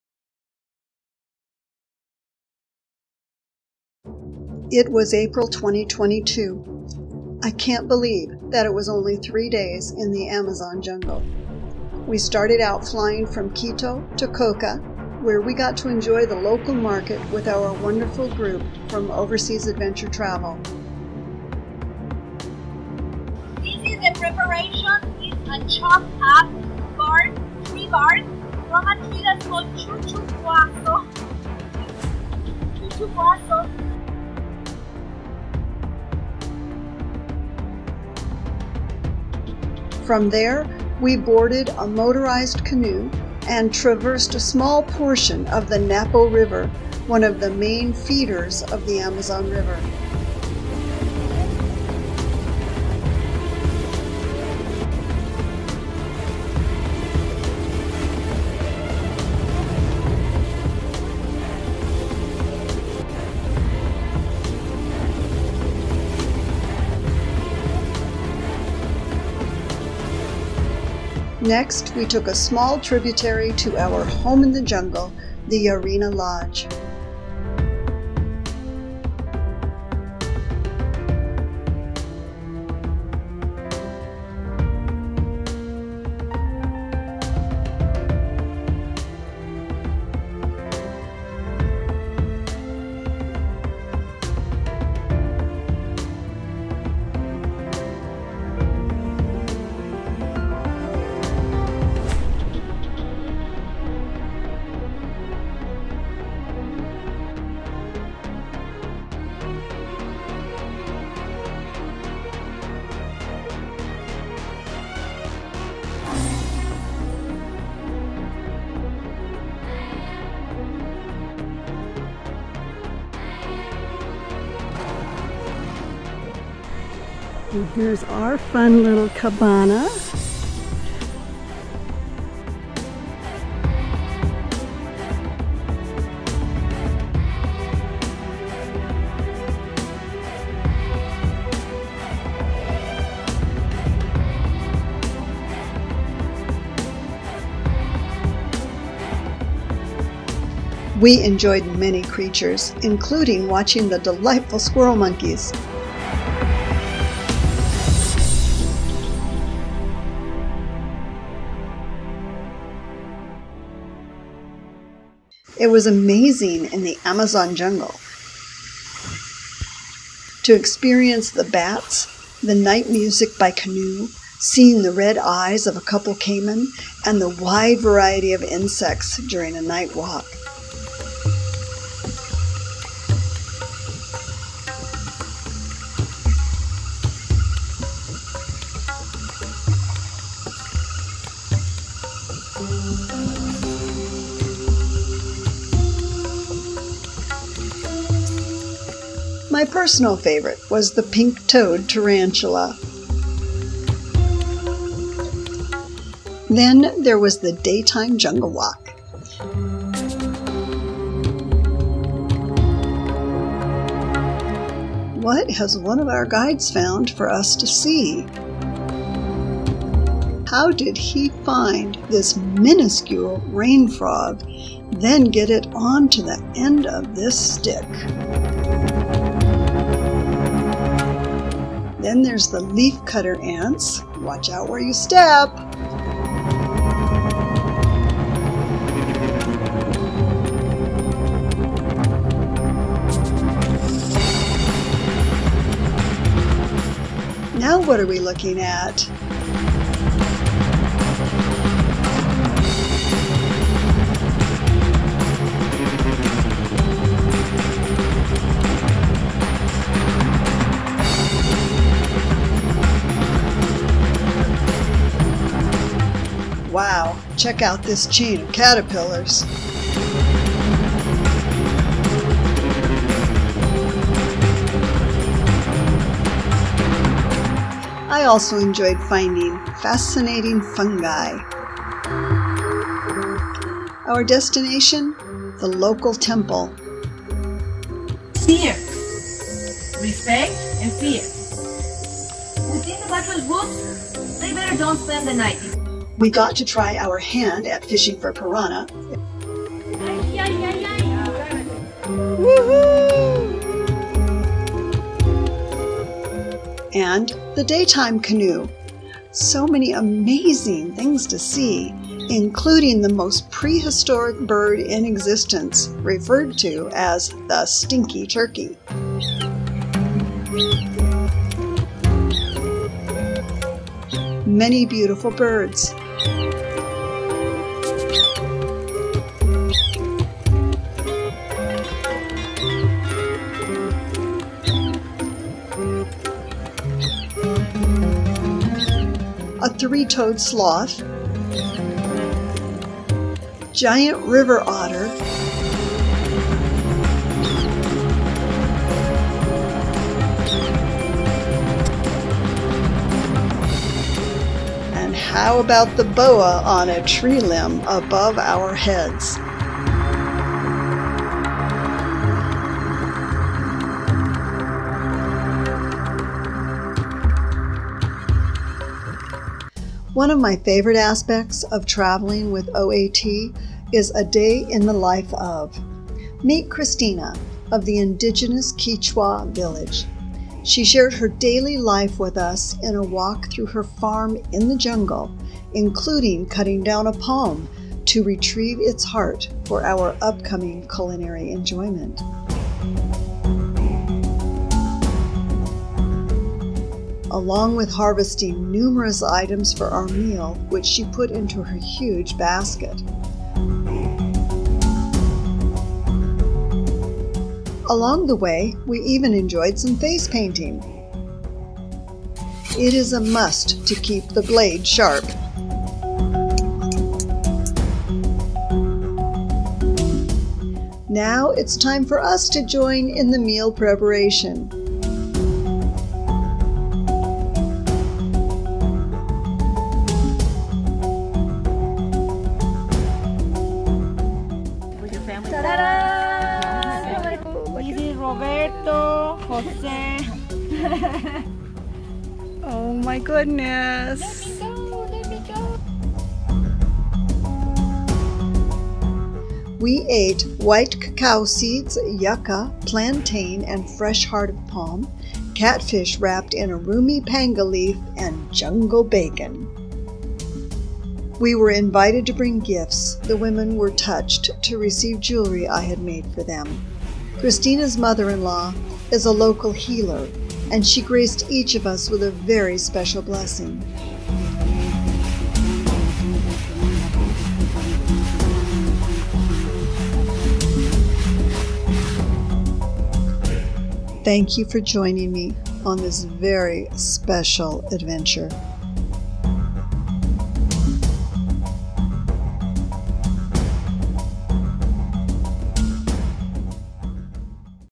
Amazon Jungle Ecuador - Overseas Adventure Travel
This is an 8-minute story of 3 days in the AmazonJungle as part of our trip with OverseasAdventureTravel #'UltimateGalapagos&Ecuador'sAmazonWilds'. You will see night and daytime canoe trips, jungle trails, variety of animals, insects and birds, and one of my favorite experiences - ADayinTheLife... of a Kichwa family.
I used my new GoPro10 for many of the video clips and my GalaxyS10 for other video clips and still shots. Several photos were contributed by our group leader and group members.
Amazingly Cool!!  Enjoy - I sure did and am thrilled to share my experience with you.  4/2022